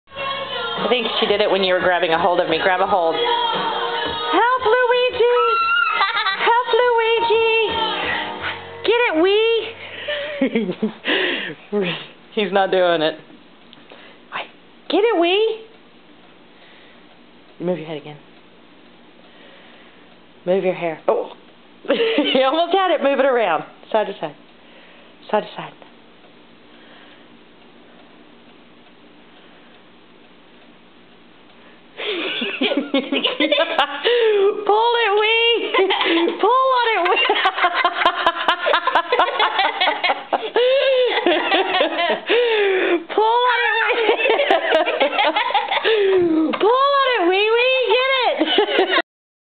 Pulling Hair
Luigi playing tug of war with Ji Ji's hair